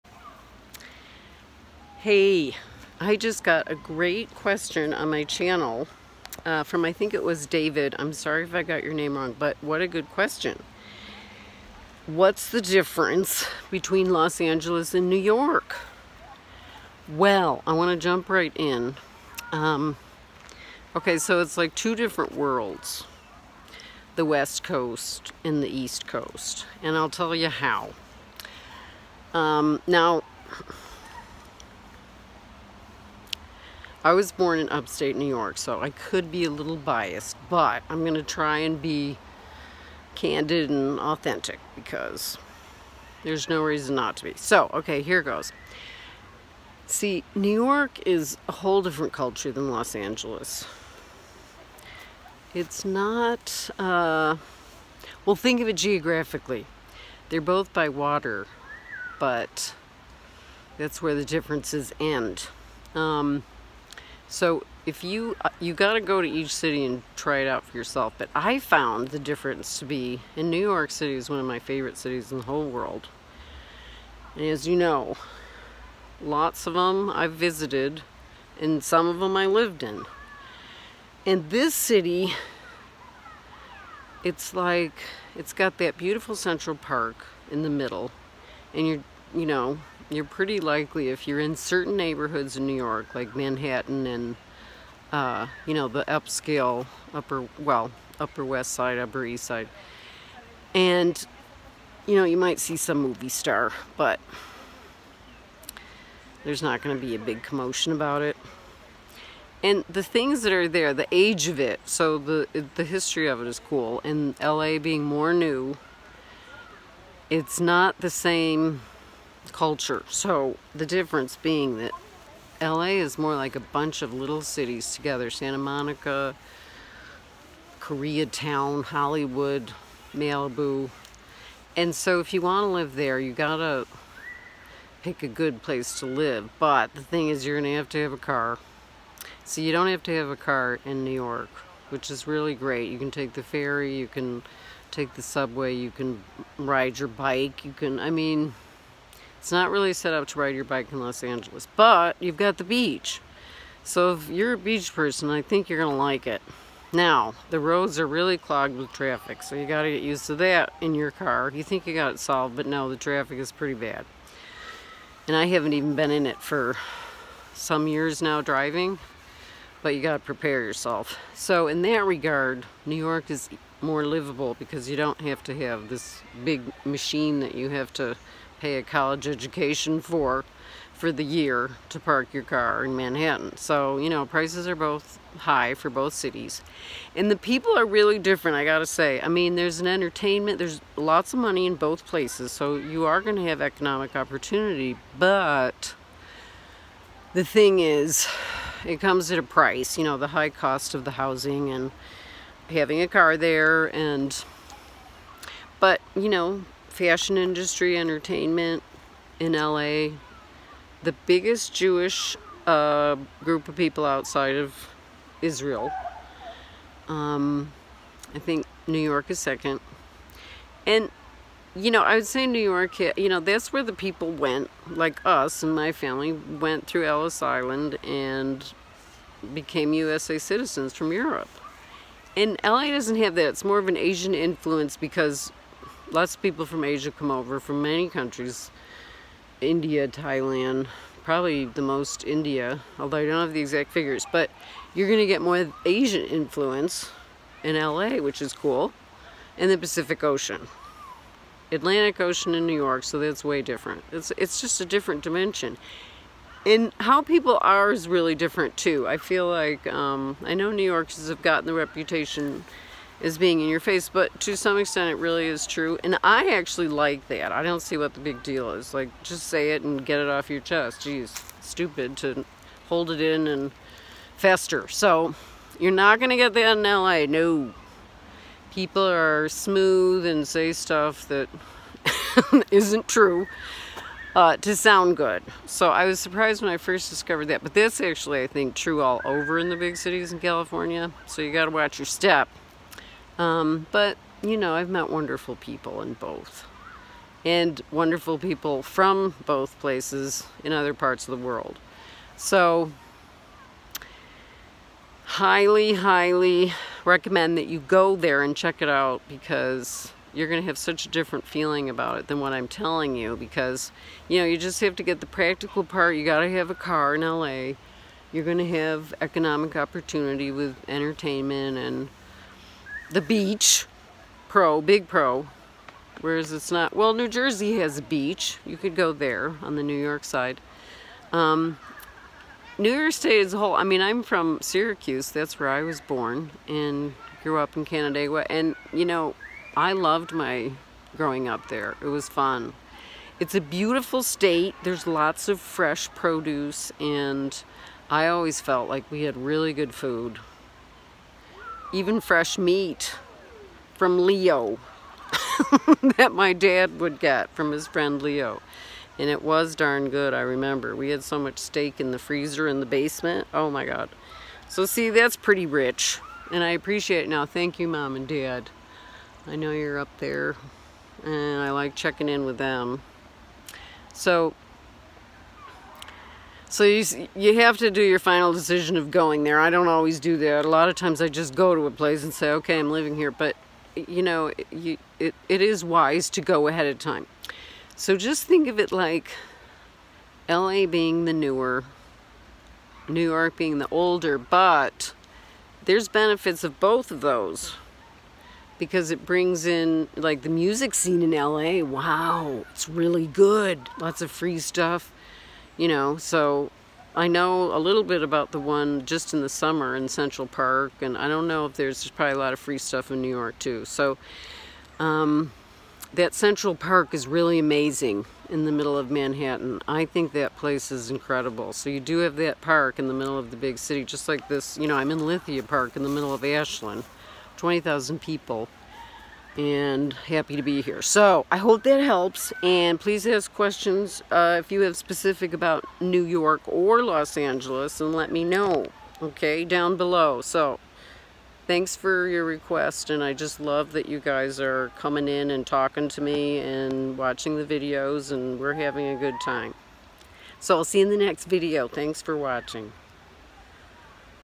Los Angeles vs New York City
Do you wonder about which city is a better choice for you? Check this out to consider the pros and cons before moving to the West Coast or the East Coast. Best thing to do is to visit Los Angeles or New York City before deciding but this video helps in the process.

Join Mary over on INSTAGRAM:
marybartnikowskiphotography

marybartnikowskiphotography